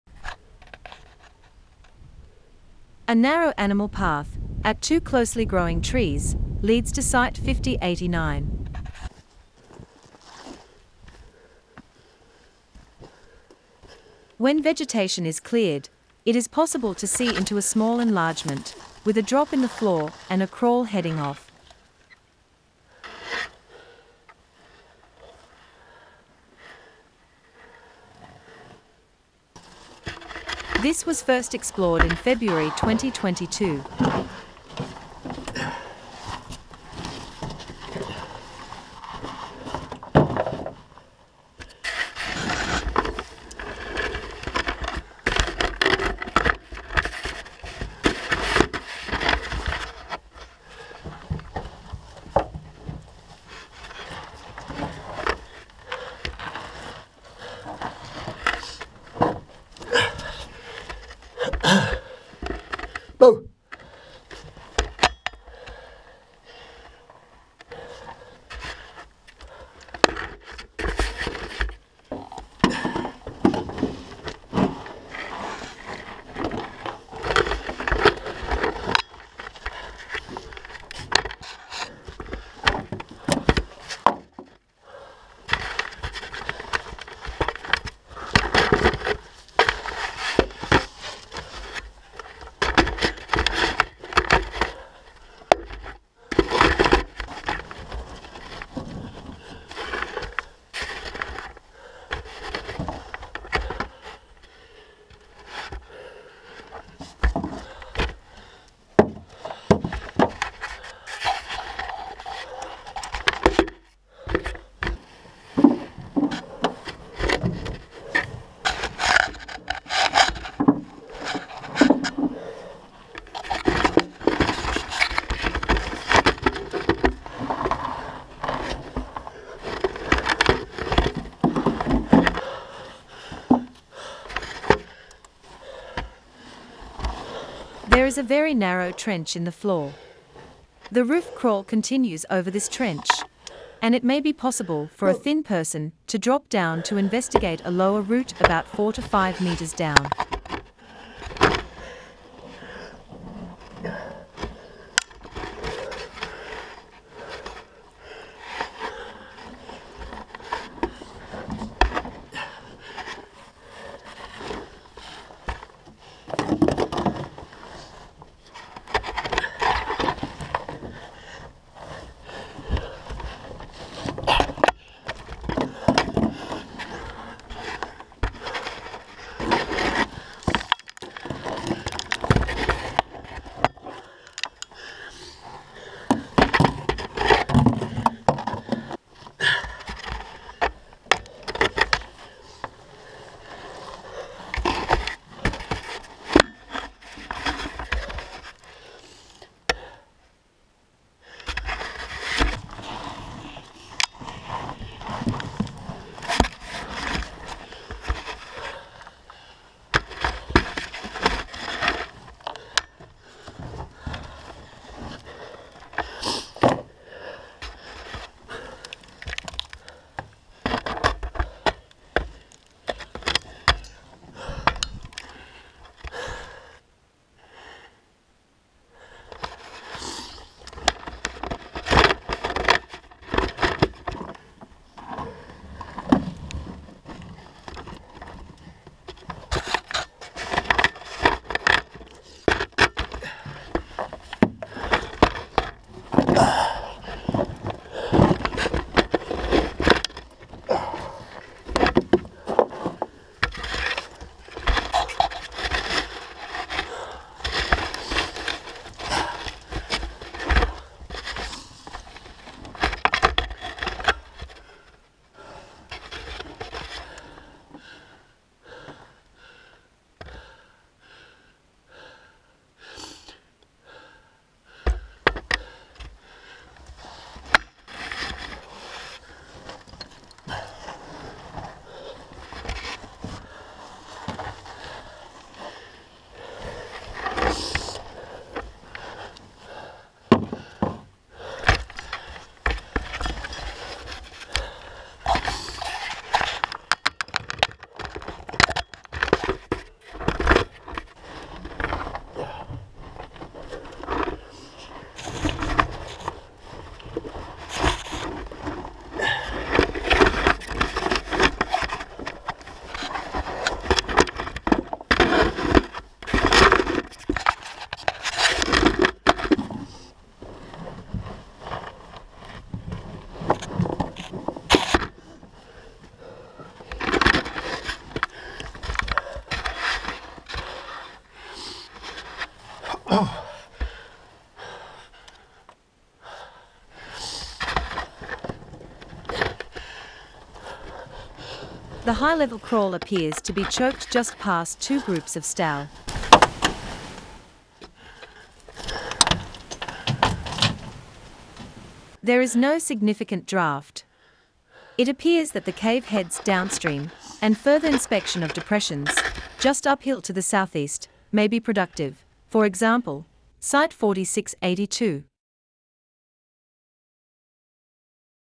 Cueva de los Dos Árboles
A crawl to the end of this short cave.